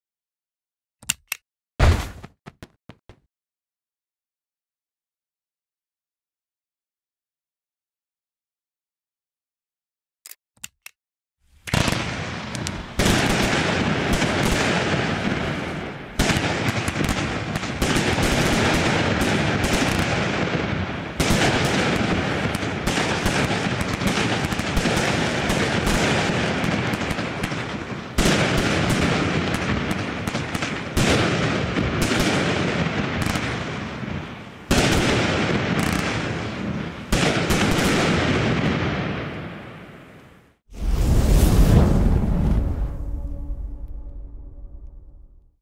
Pyratenschatz von Röder Feuerwerk - Produktvideo
Neue Mini-Showbox von Röder Feuerwerk mit abwechslungsreichen Effekten und Schussmustern